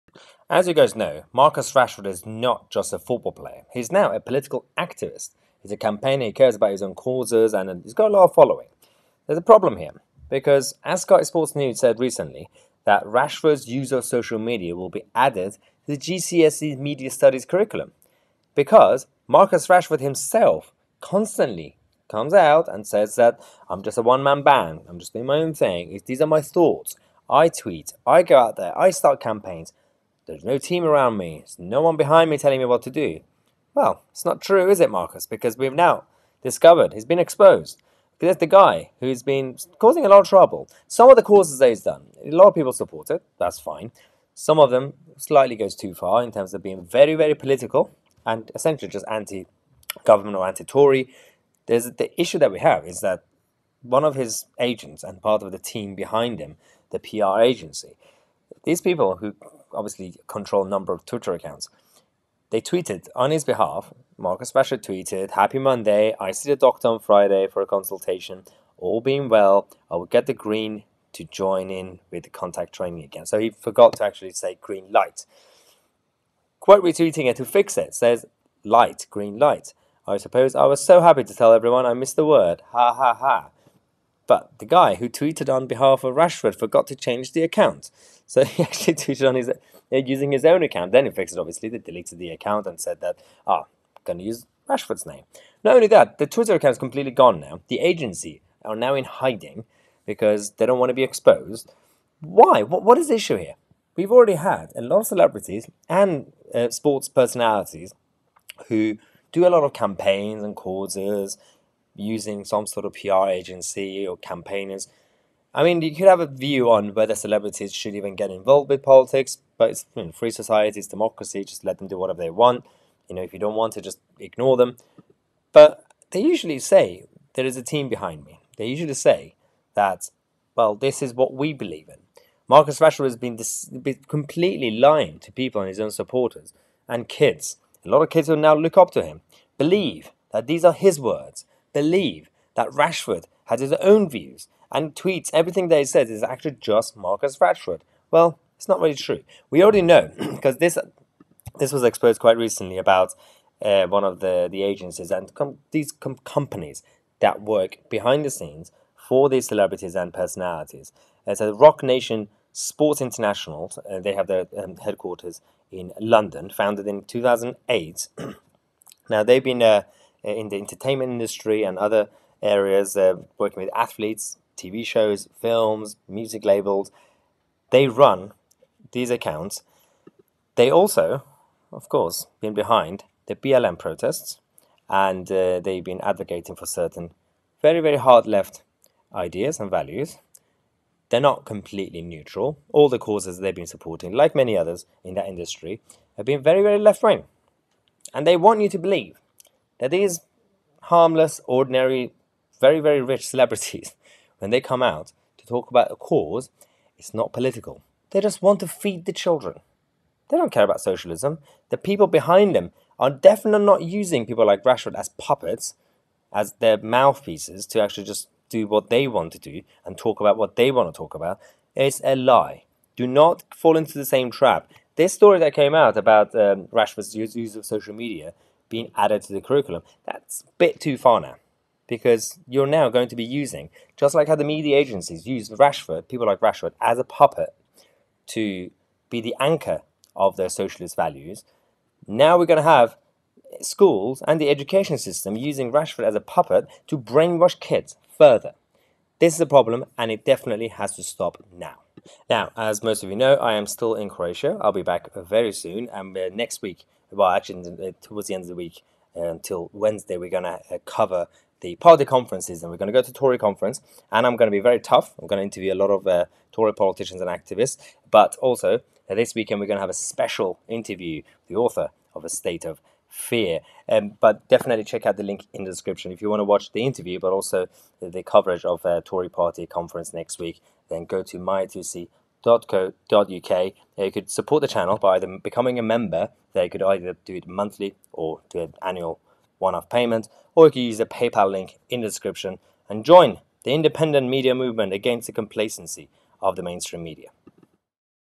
Rashford's Real Bosses Behind His Political Activism
Ever since Rashford became political, he has been selling himself as an ordinary guy who only cares about compassion. He claims he is his own man and a one man band. That's not really true, is it?
The people running the show are not Rashford and his own brain. This video exposes the agency behind his political campaigns, from free school meals to universal credit. Just another millionaire celebrity telling us how to live our lives.